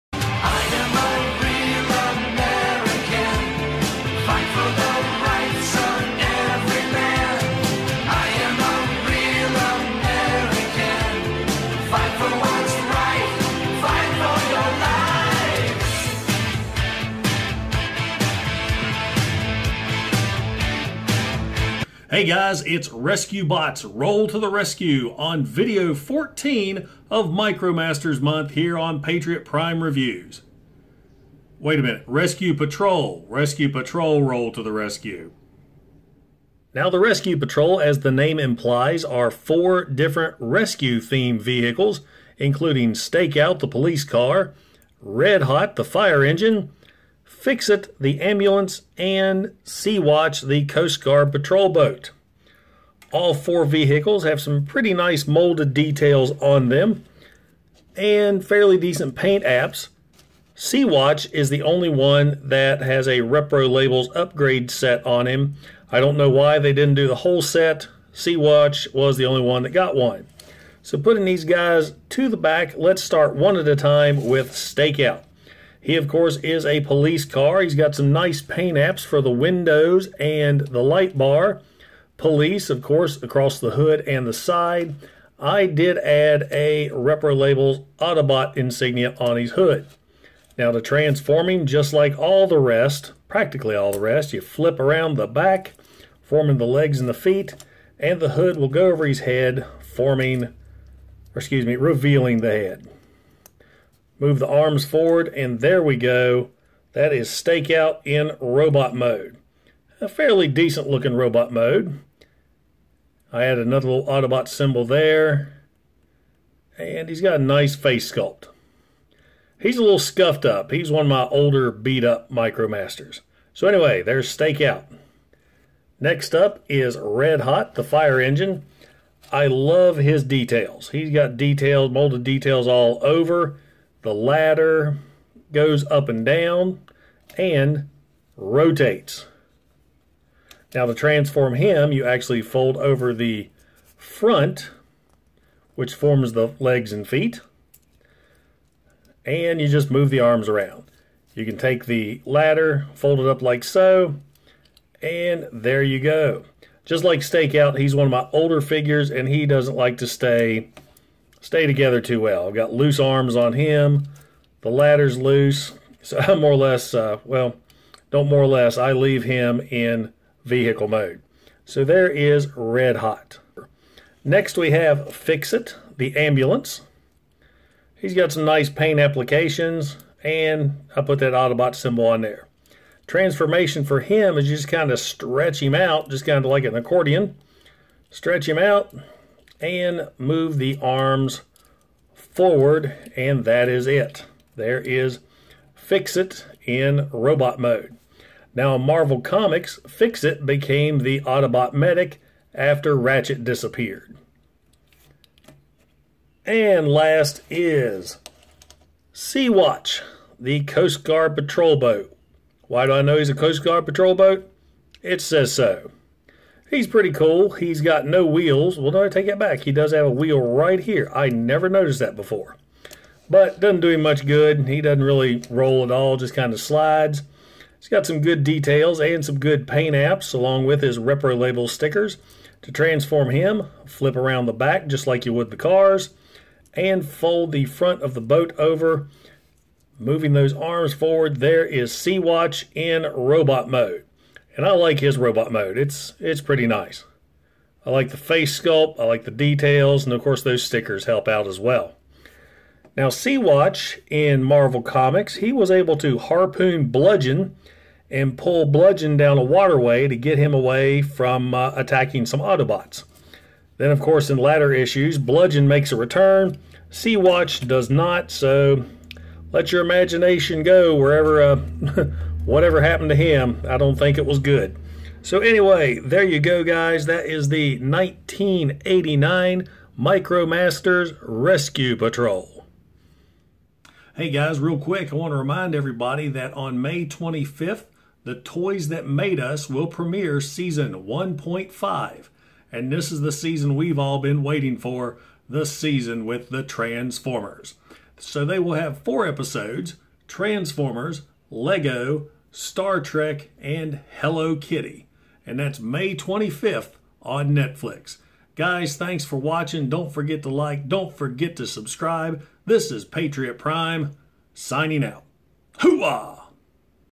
Micromasters Month: Review # 14 - The Rescue Patrol
May is Micromaster Month here on Patriot Prime Reviews!
All this month I'll be looking at some of the different Micromaster Patrols, Squads, and Bases from the the tail end of G1.
This Review will cover Micromaster RESCUE PATROL.